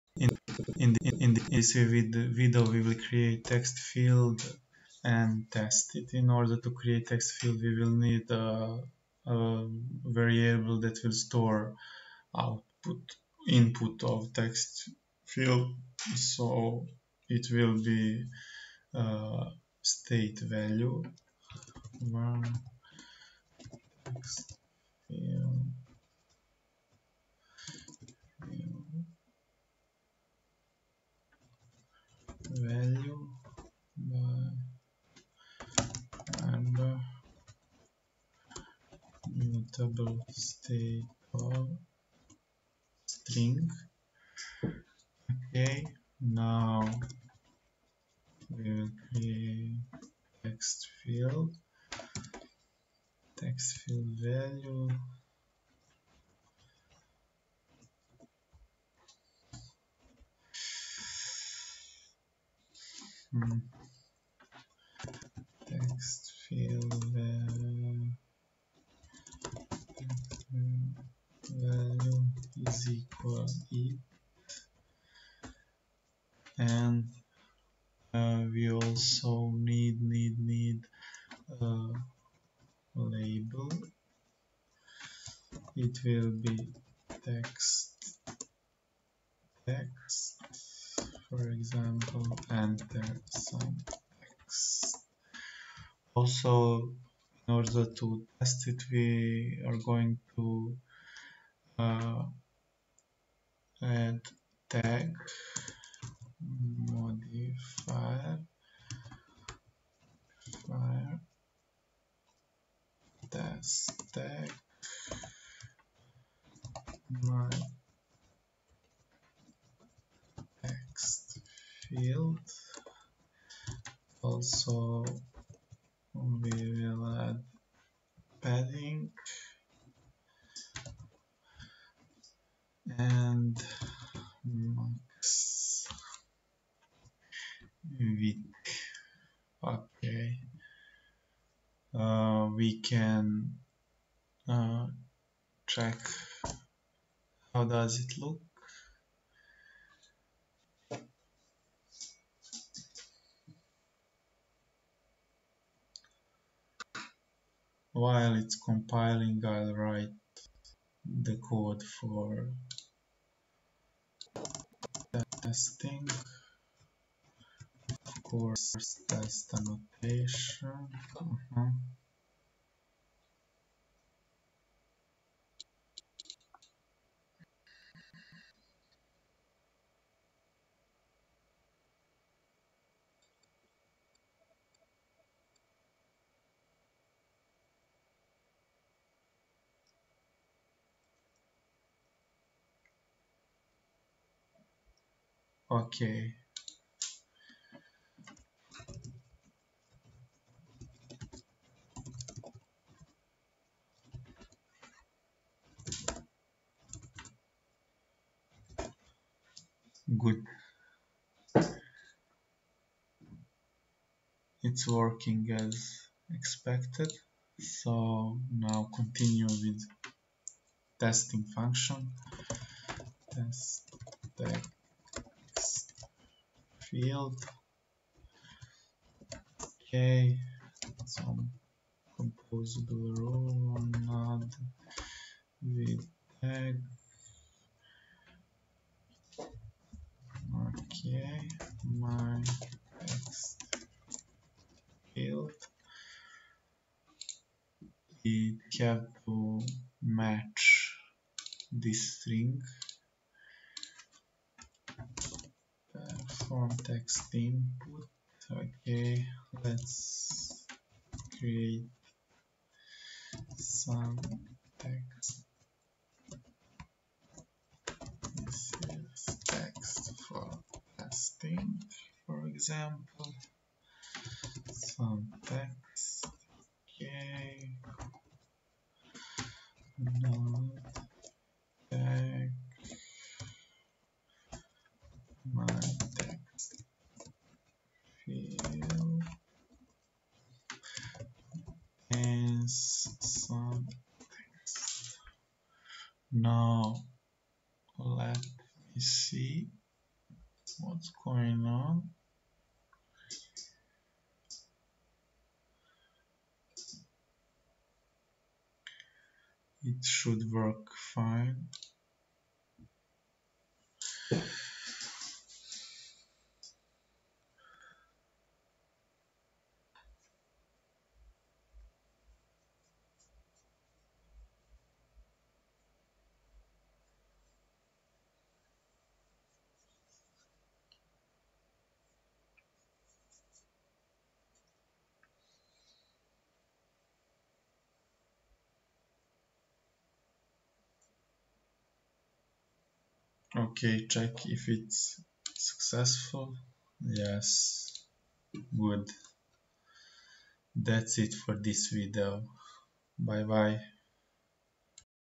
TextField, UI Test - Android Jetpack Compose #3
In this video we are going to create TextField and test it.